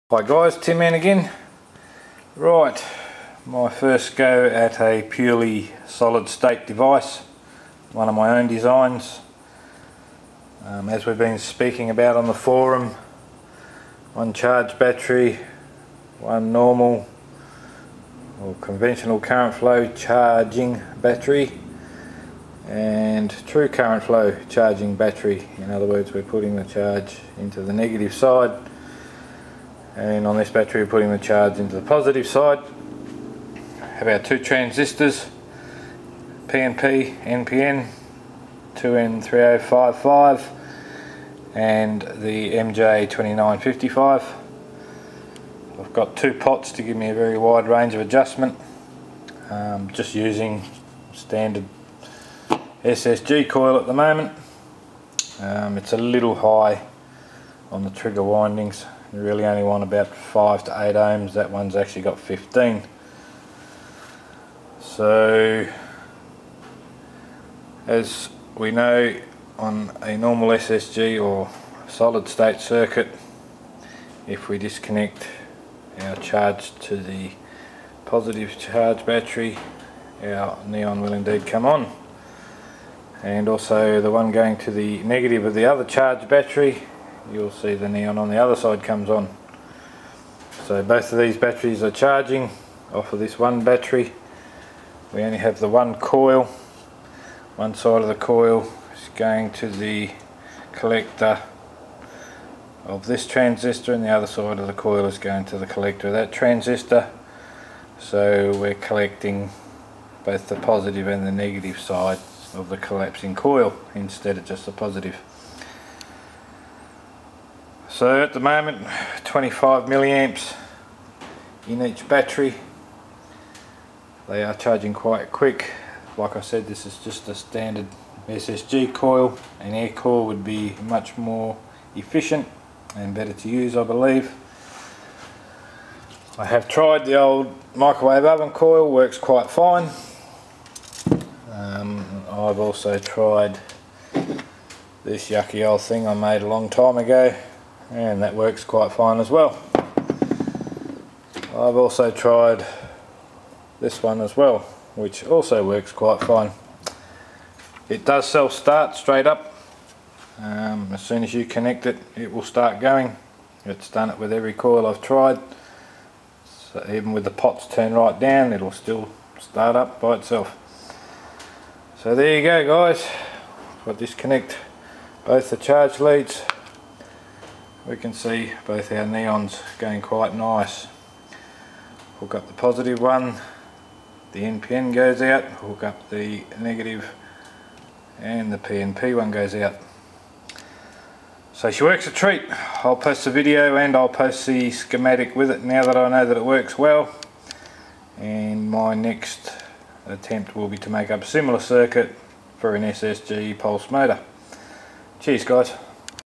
SS-SSG with reverse charge.flv
Ok this circuit i designed catches both the positive and negative backEMF of the collapsing coil in a ss charger.This allows you to charge 2 batterys of the one run coil and battery.